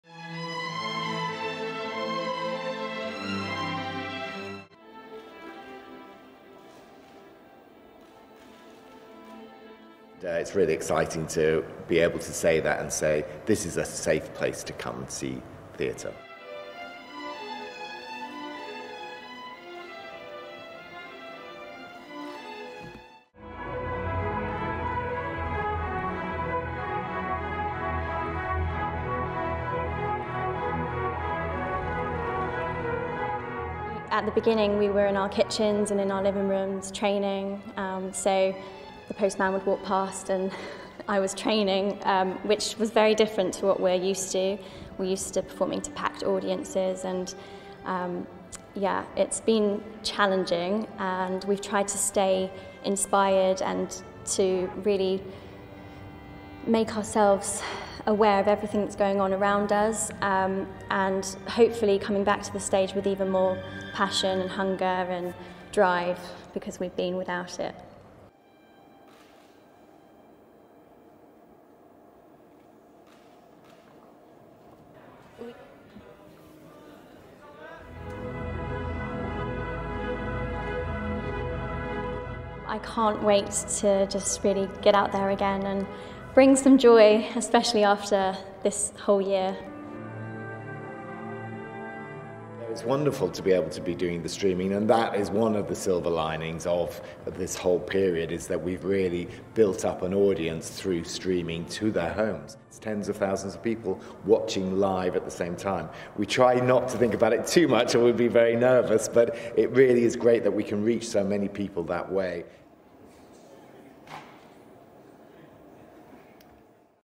Royal Ballet dancers prepare for re-opening
After five months, the Royal Ballet welcomes back audiences to the Royal Opera House on Tuesday with "21st Century Choreographers".